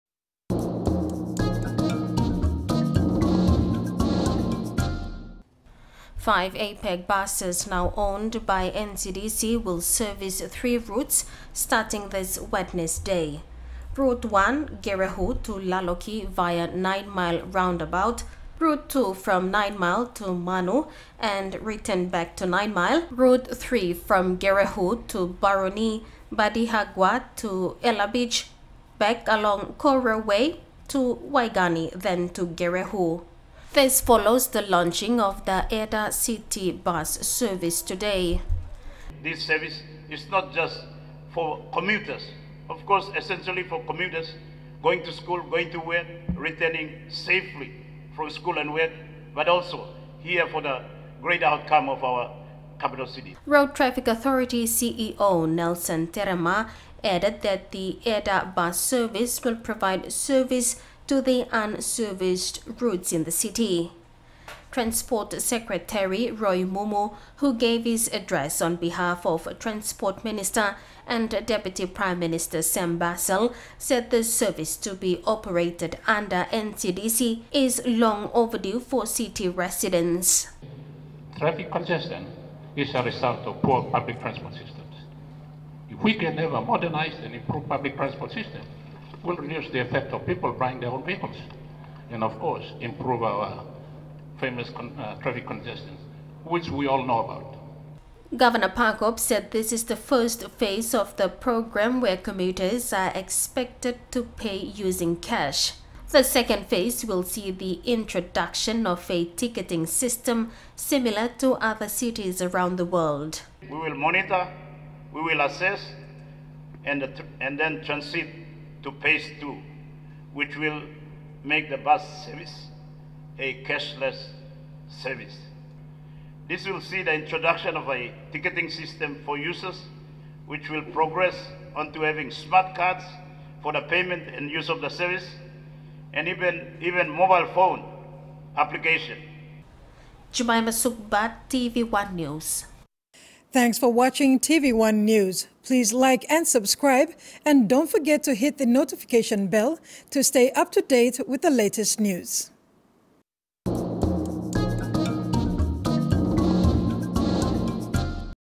Eda City Bus Service
NCDC will provide convenient transportation for commuters within seven new routes in Port Moresby following the launch of the Eda City Bus Service today.

The Capital city’s Governor Powes Parkop said NCDC has been in discussion with relevant stakeholders for a long time to provide this service.

He added that the service provides a reliable transport system for city residents.